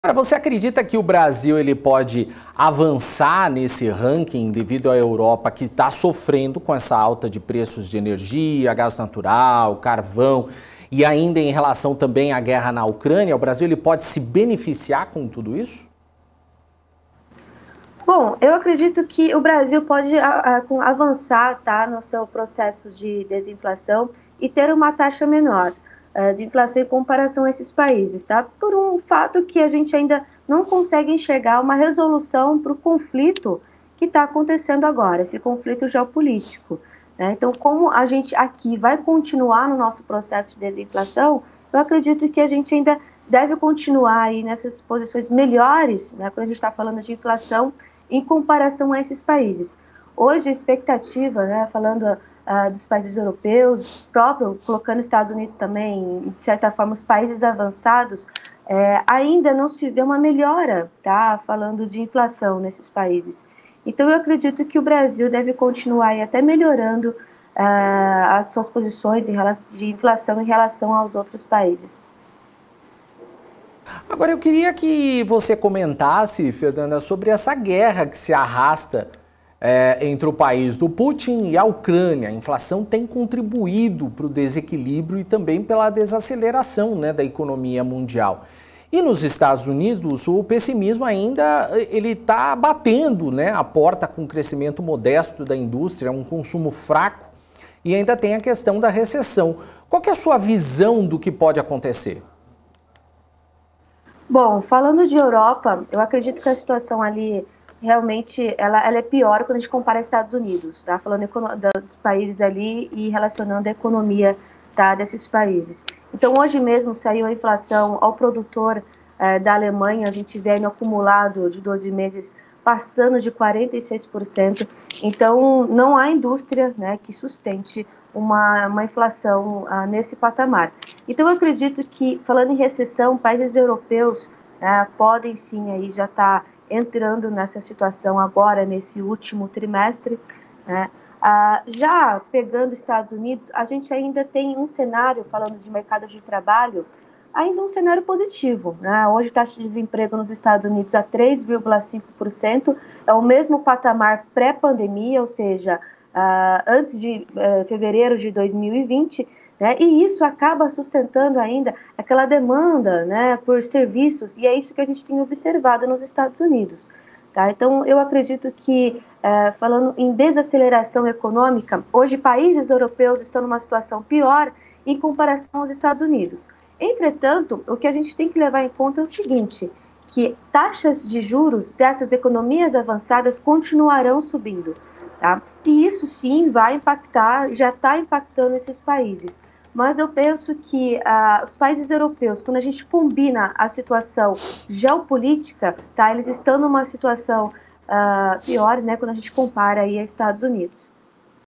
INFLAÇÃO: BRASIL PODE SE BENEFICIAR COM EUROPA E GUERRA?
Fernanda Mansano, economista e professora do IBMEC comenta sobre como o a economia brasileira pode se sair melhor em relação a  outros países